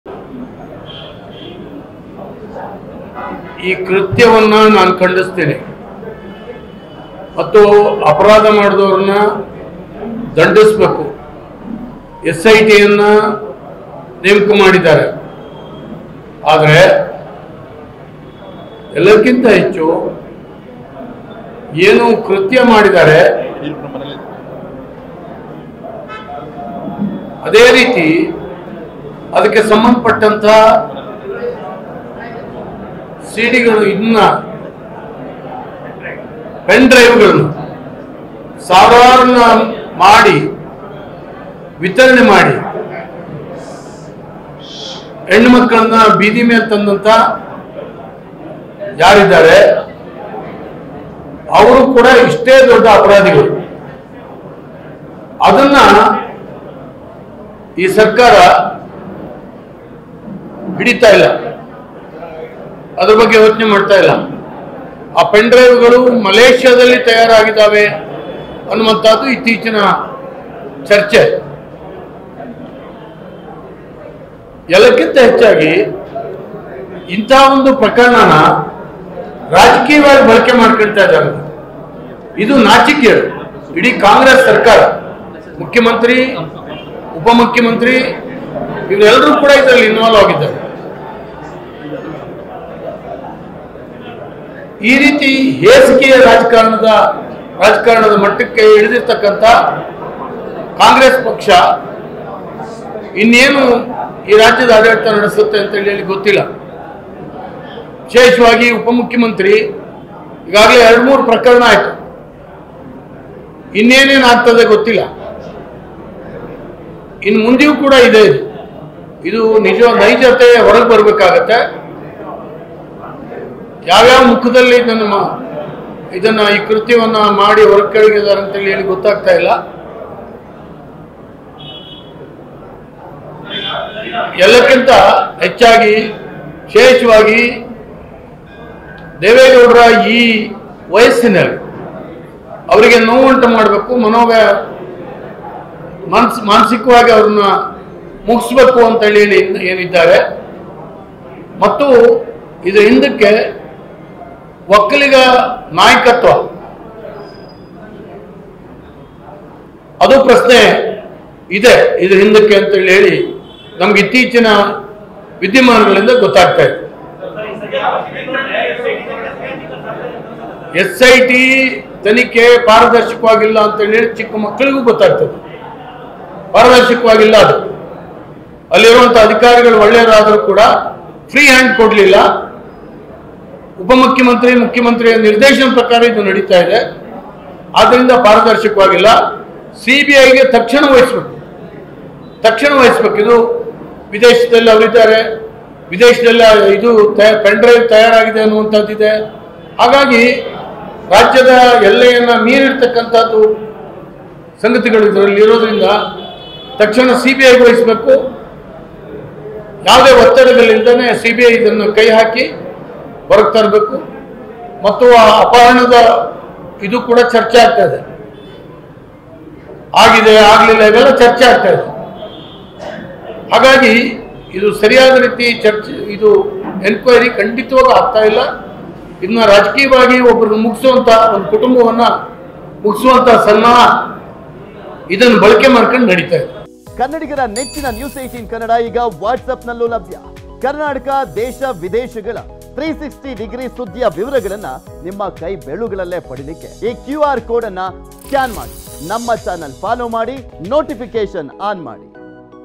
ಪ್ರಜ್ವಲ್ ಕೃತ್ಯವನ್ನ ಖಂಡಿಸ್ತೇನೆ, ಅಪರಾಧ ಮಾಡಿದವ್ರನ್ನ ಖಂಡಿಸಬೇಕು | Prajwal Revanna Video | HD Revanna
ಪ್ರಜ್ವಲ್ ಕೃತ್ಯವನ್ನ ಖಂಡಿಸ್ತೇನೆ, ಅಪರಾಧ ಮಾಡಿದವ್ರನ್ನ ಖಂಡಿಸಬೇಕು | Prajwal Revanna Video | HD Revanna | N18V

News18 Kannada Live | ನ್ಯೂಸ್ 18 ಕನ್ನಡ ಲೈವ್
Karnataka | ಕರ್ನಾಟಕ
Kannada | ಕನ್ನಡ